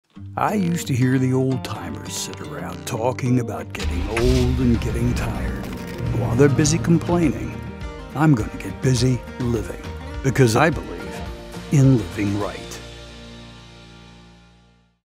Joe's Tire - Live Your Life - 15 TV Commercial 2021
Joe's Tire 2021 TV Commercial, Live Your Life is one of our super bowl LVI (56) commercials. A story about choosing not to get old and tried but as you age you choose to live life with the people you hold dear.